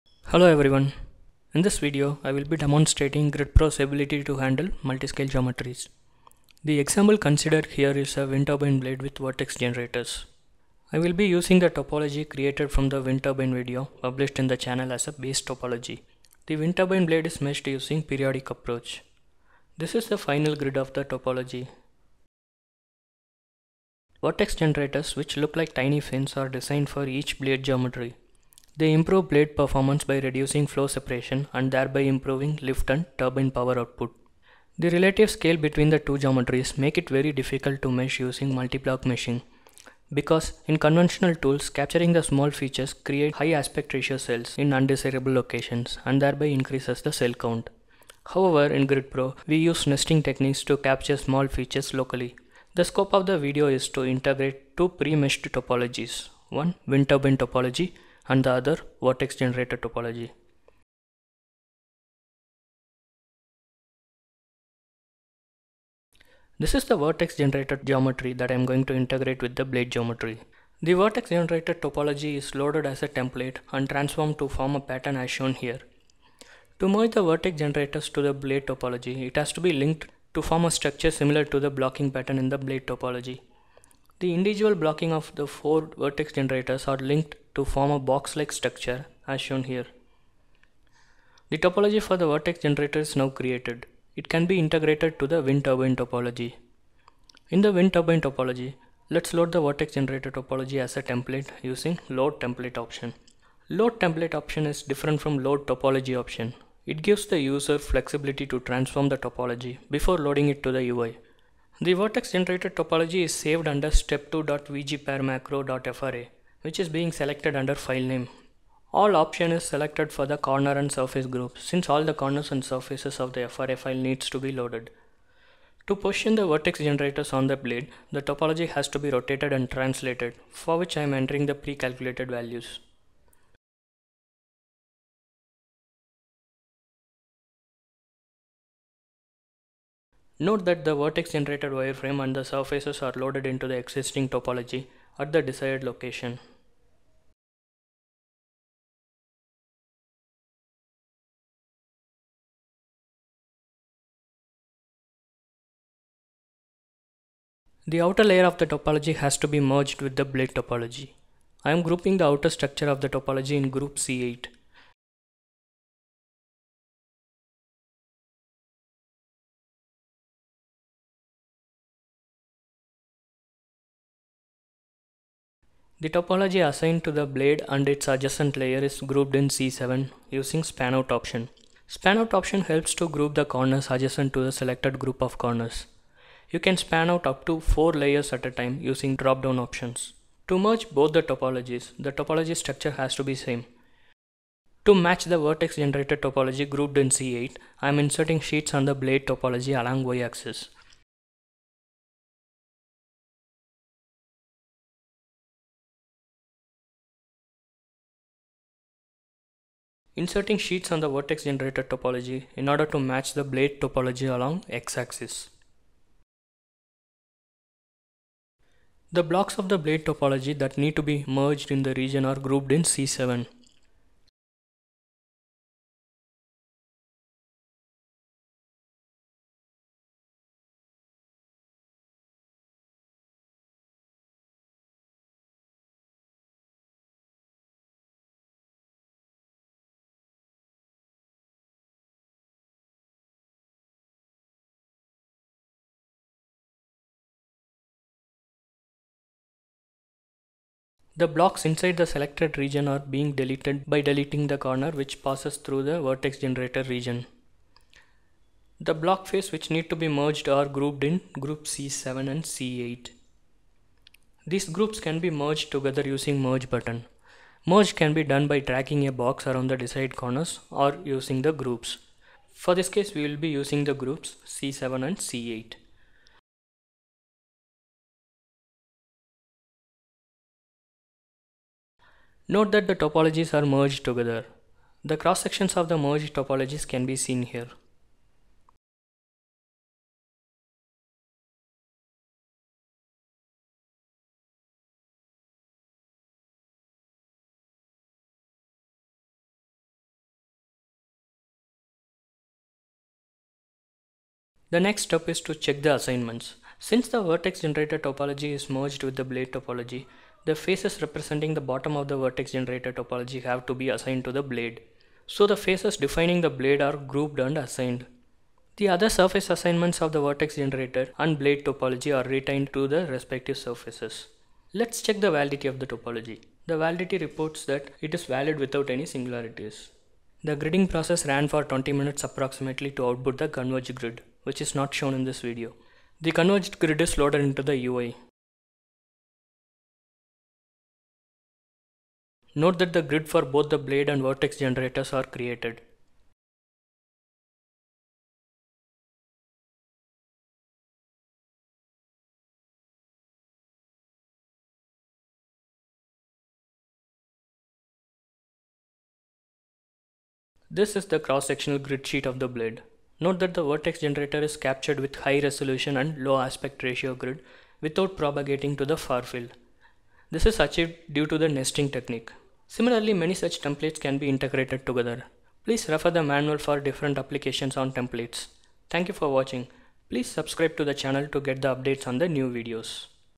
Vortex Generator meshing (Hexa)
This video demonstrates the GridPro's ability to handle multi-scale geometries. The nesting feature is used here to capture small features like vortex generators (VG) on a large scale wind turbine blade.